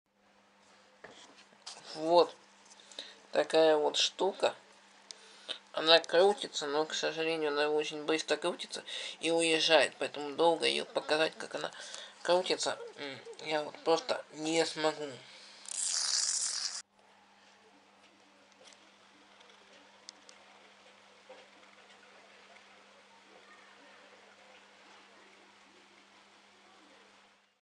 Коліщатко
колесо від мишки відмінна дзига